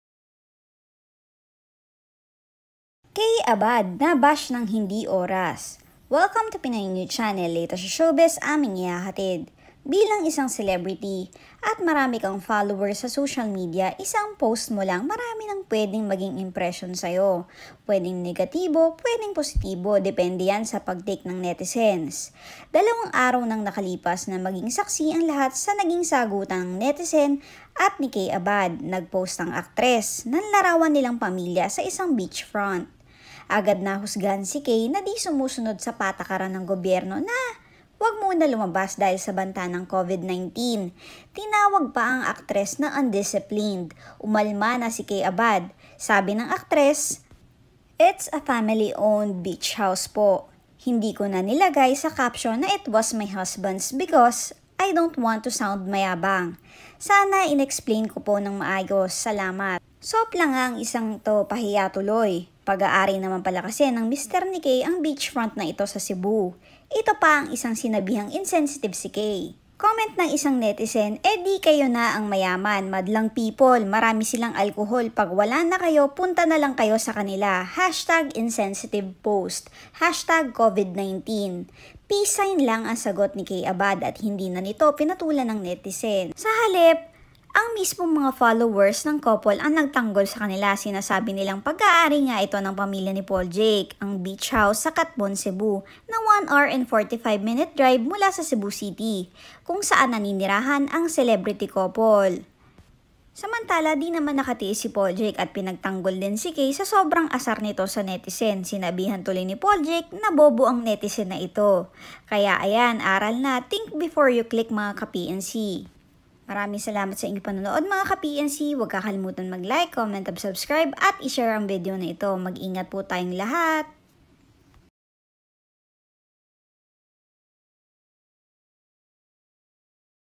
Kaye Abad at Paul Jake binatikos matapos mag post ng kanilang Quarantine na pang yamanin! Mayabang?
Eh ano magagawa natin kung ganun talaga ang buhay nila.. 😑